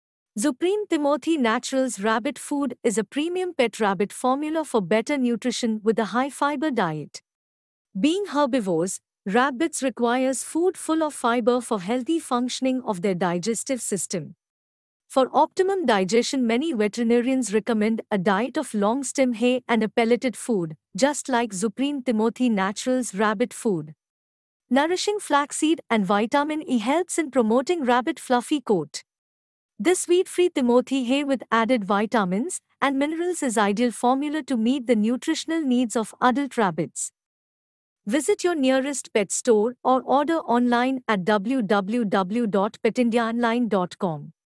Zupreem Timothy Hay
Zupreem nature's timothy hay is essential in the daily diets of growing and adult rabbits, guinea pigs, chinchillas, and other small animals. This low-protein, long-strand fiber hay is the most recommended hay for everyday feeding.